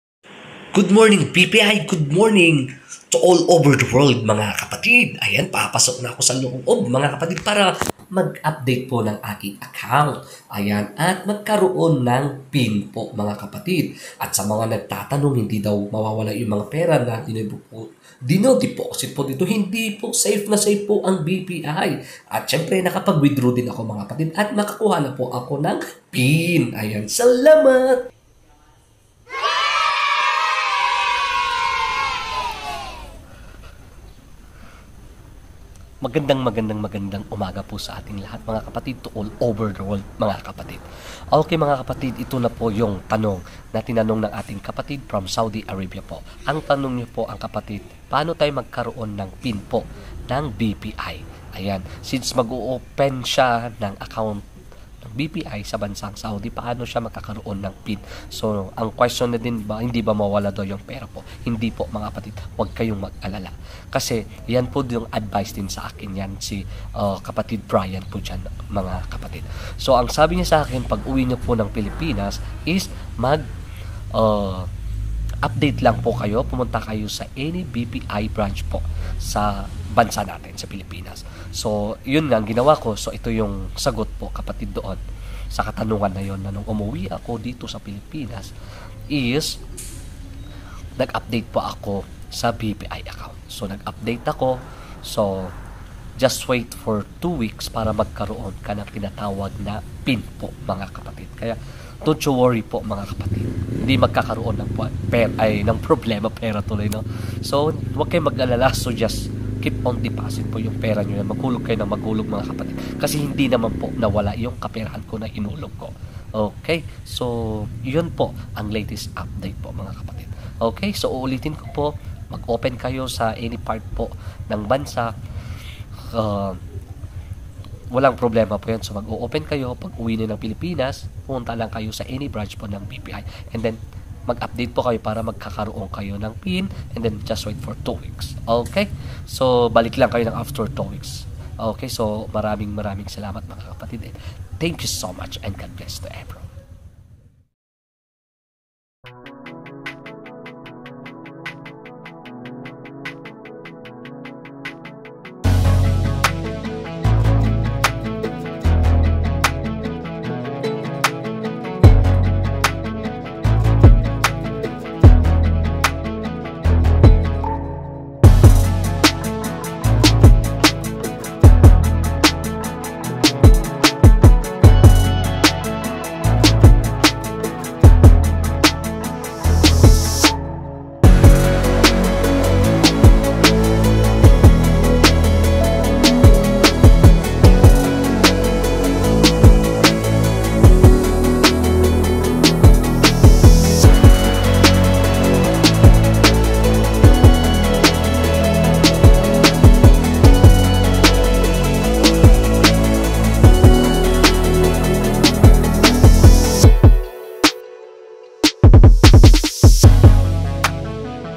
BPI UPDATE
UPDATE NG BPI BANK ACCOUNT PARA MAGKAROON NG PIN!